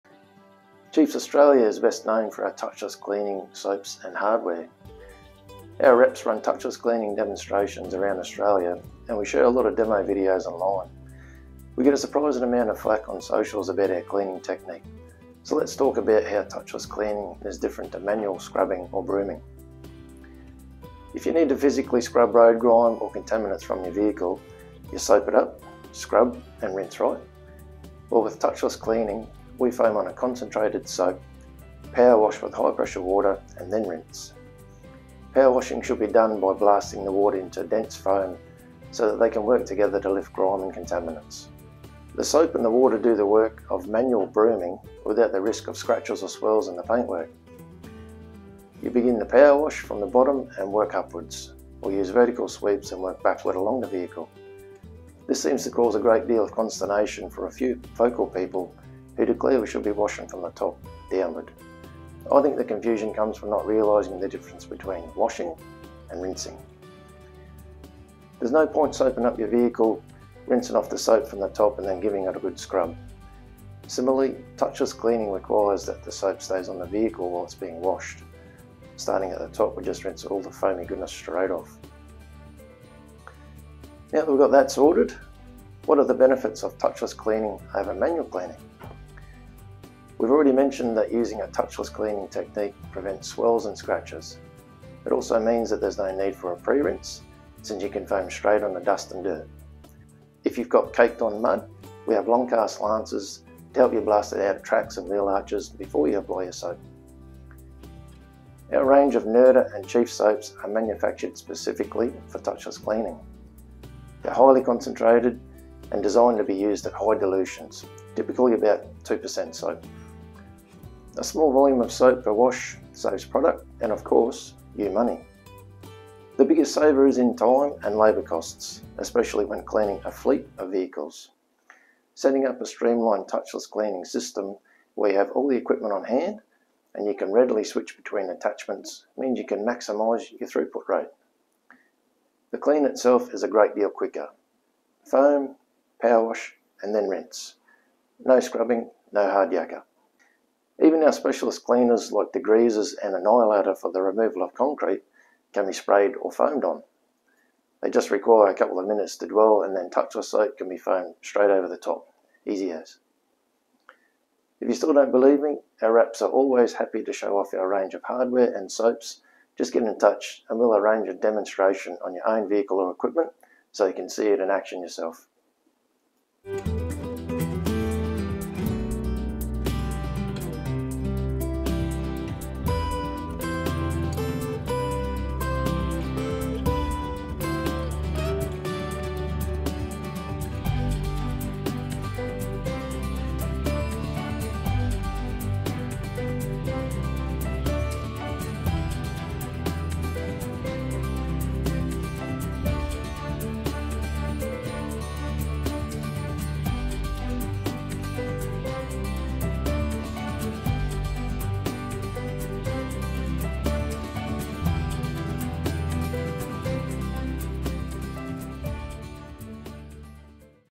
Touchless Cleaning – What's with Washing From the Bottom?
We get a whole lot of flack on social media about our touchless cleaning technique.
Yep, we clean from the bottom. Here's why.